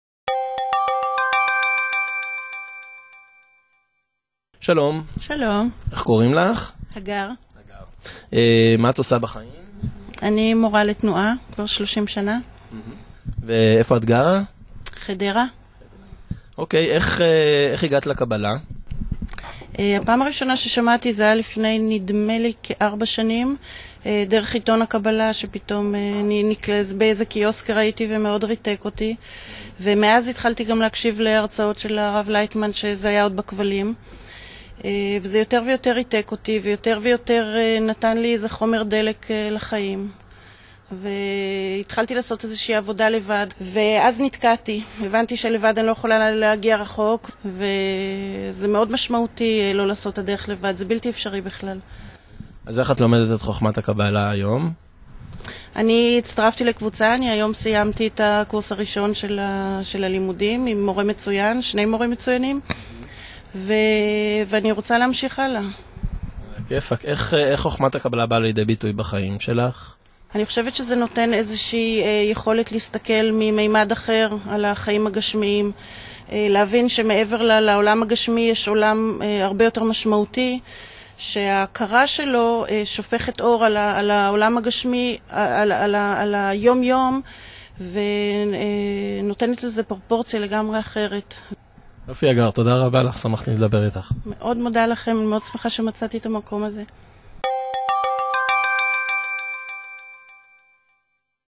בגובה העיניים הגר חומר דלק לחיים
הגר, מורה לתנועה מחדרה, נתקלה בחכמת הקבלה דרך העיתון, מאז היא לומדת בקמפוס קבלה, לימודים שנותנים לה חומר דלק וטעם לחיים